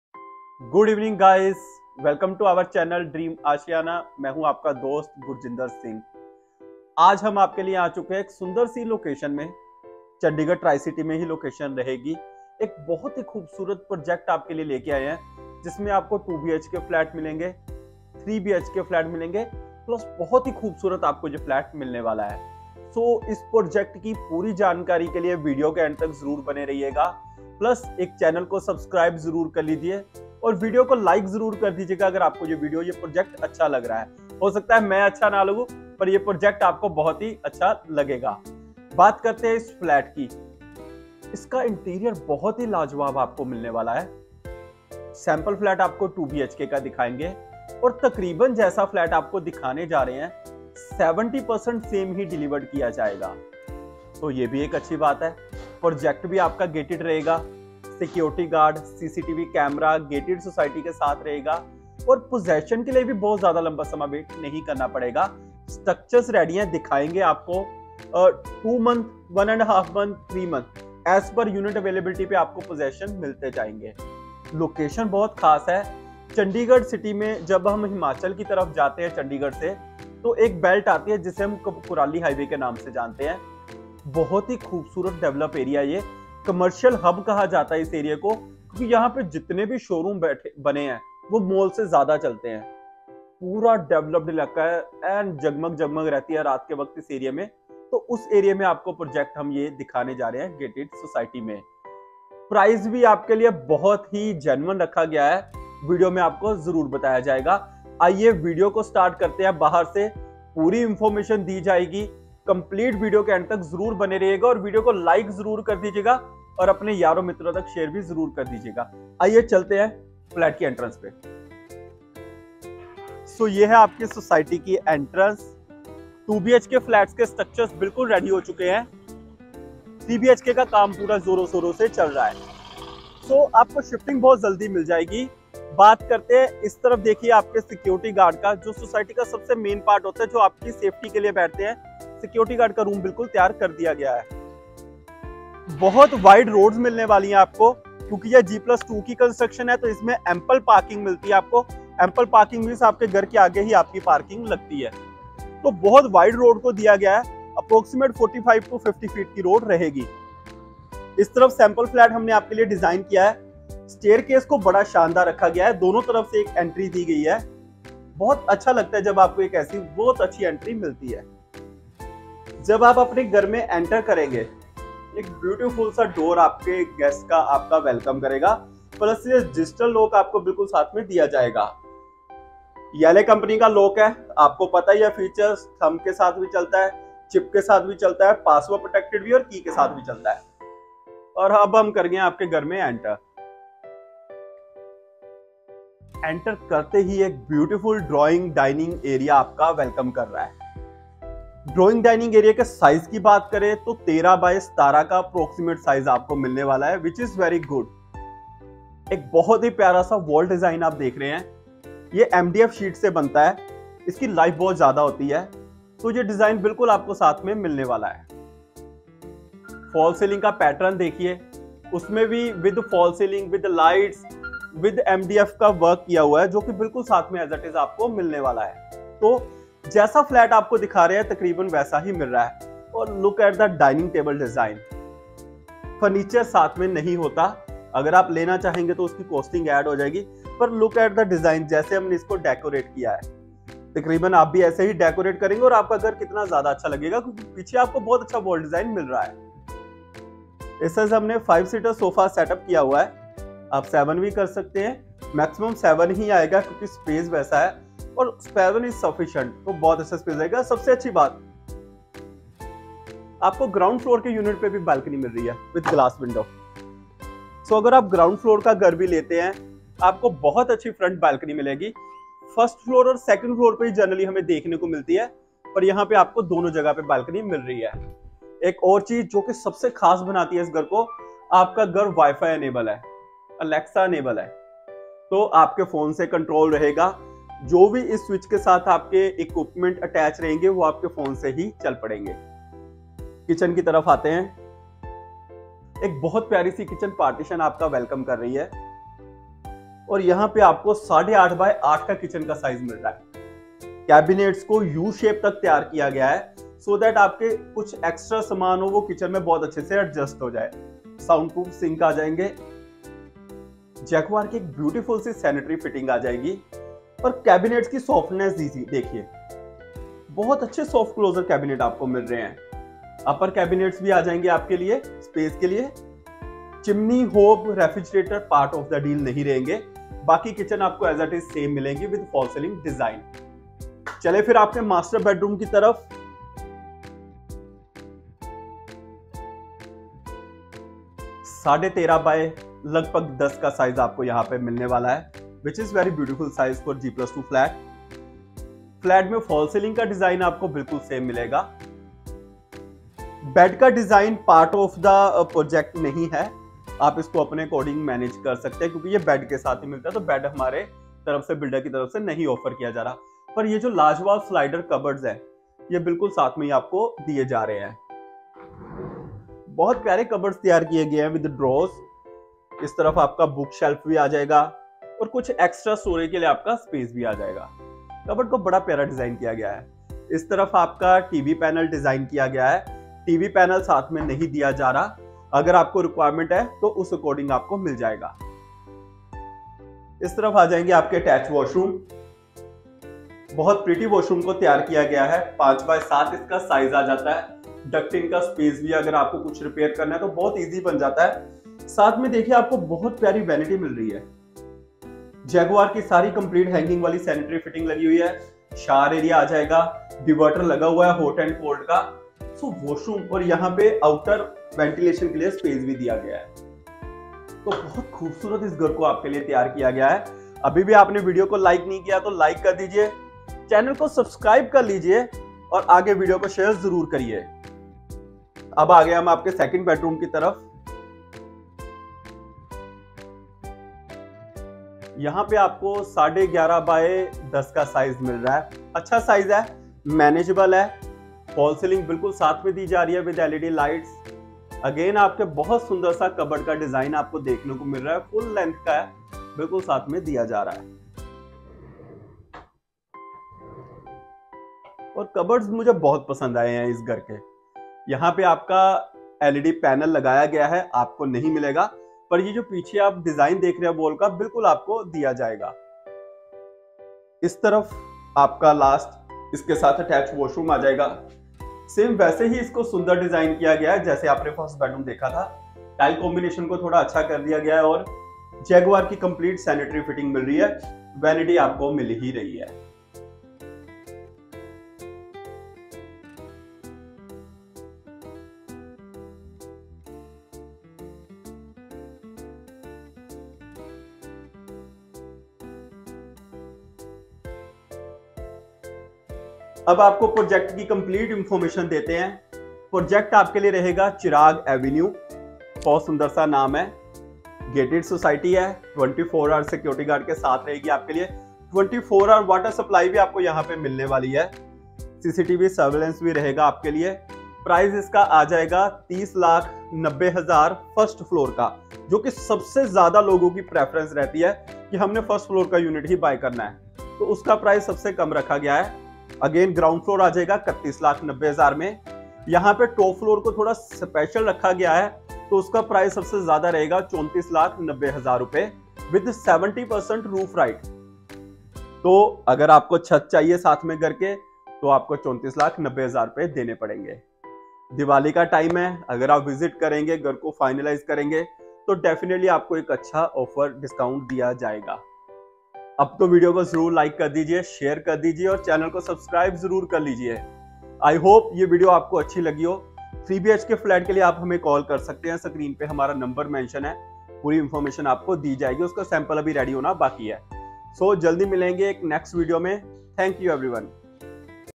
2BHK Modern Design Flats With Smart Features With Beautiful interior in Chandigarh Tricity.
Amandeep Kaur

This is Good Project With 2BHK & 3BHK Flats With Smart Feature with Attractive interior in Greater Mohali in Chandigarh Tricity.

Gated Project With Very Prime Location With all Daily need Requirements in Surroundings.

24 Hours water Supply and Security With Security Guards and with CCTV

1 km Distance from Main Highway which is known as commercial hub

Pretty Elevation Which is Very Attractive.

Attractive MDF Sheet Design in 2 Walls and False Ceiling

Smart Digital Lock System

Wifi Enable Phone acontrol

Full-size Cupboards in All Bedrooms

Smart Modular kitchen which attracts alot

False Ceiling will attract you with Led Lights and with MDF Sheet Work

Jaguar Branded Bath Sanitary Fitting

Genuine Price Watch Complete Video to know